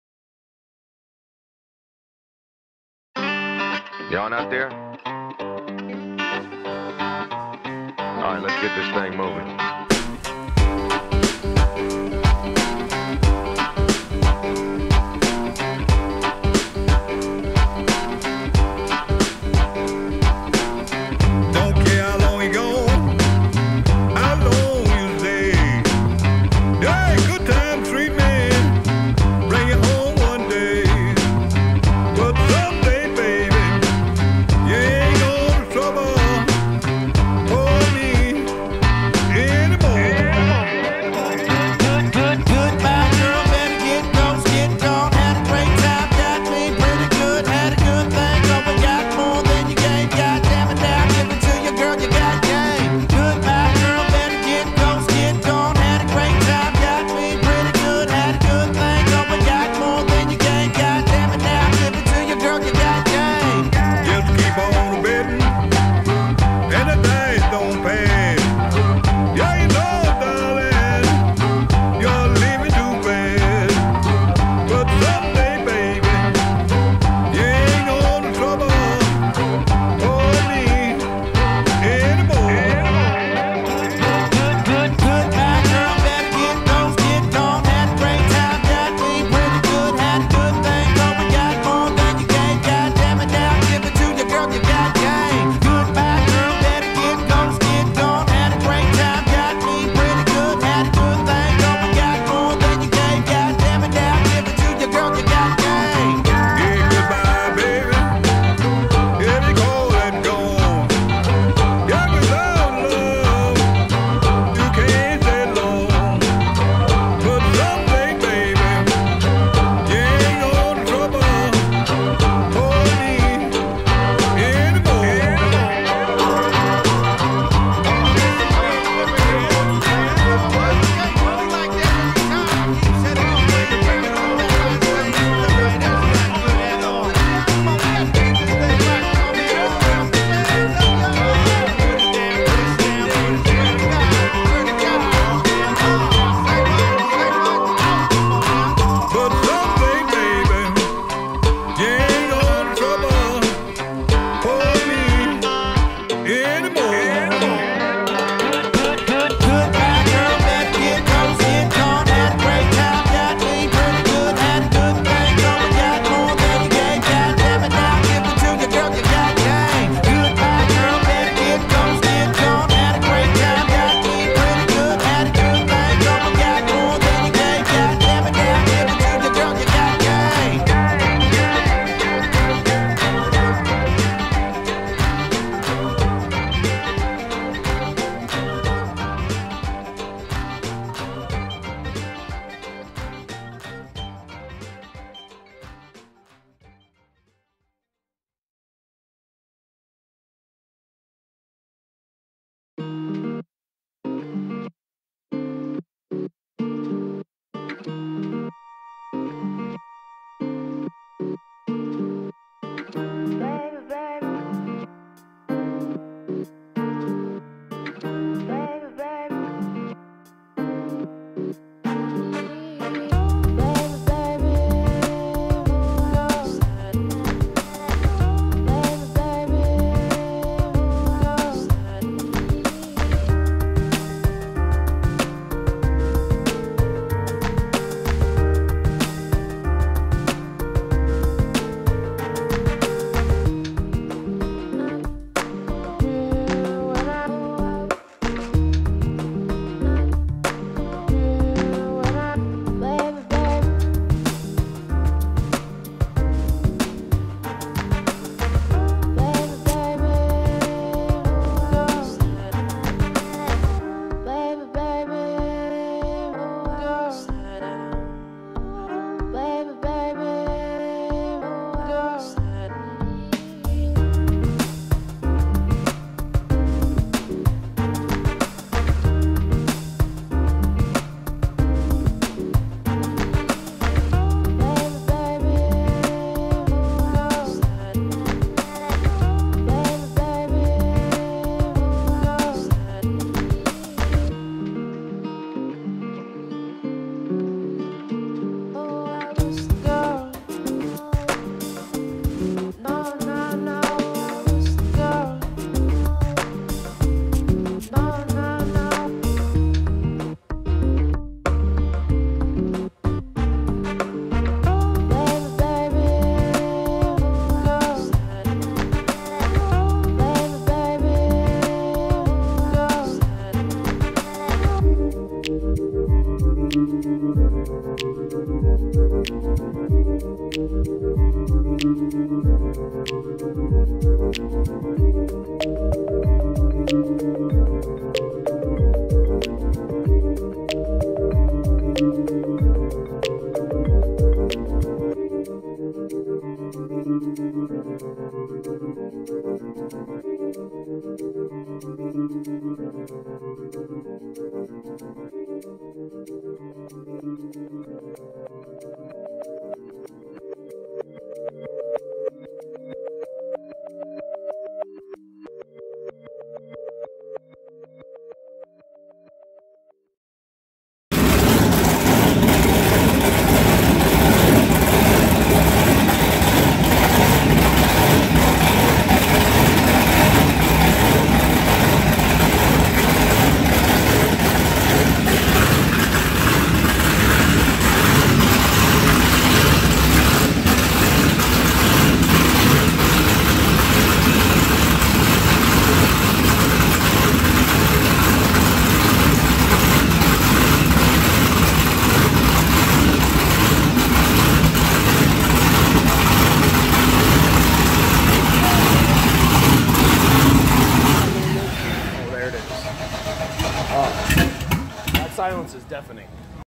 Lake of the Ozarks Shootout 2017 - pics and tunes
Just a bunch of pics with a few tunes in the background.  enjoy!!